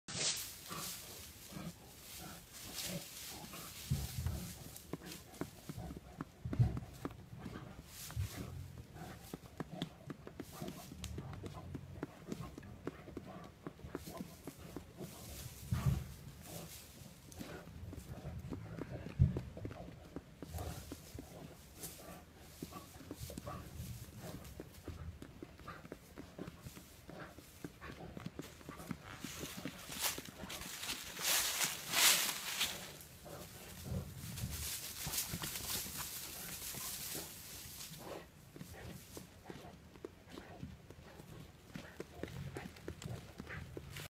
wild hog fight
- Georgia wild boar hogs fighting for breeding rights. I was able to get close enough to get the video with phone. They knew I was there but didn't care. I left after 5 min. They were still at it.